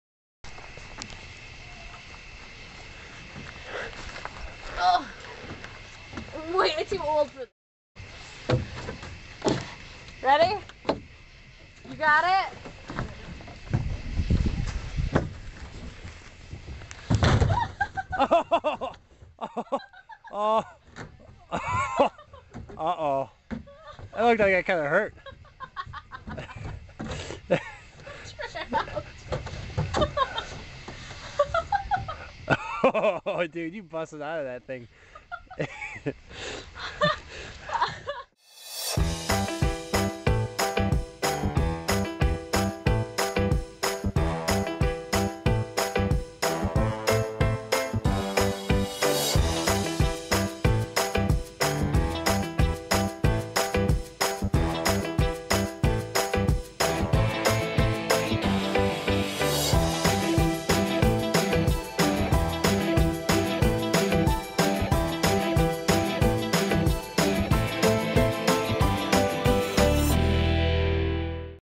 CuteWinFail: Playground Slide Fail
A kid goes down a playground slide and ends up breaking it.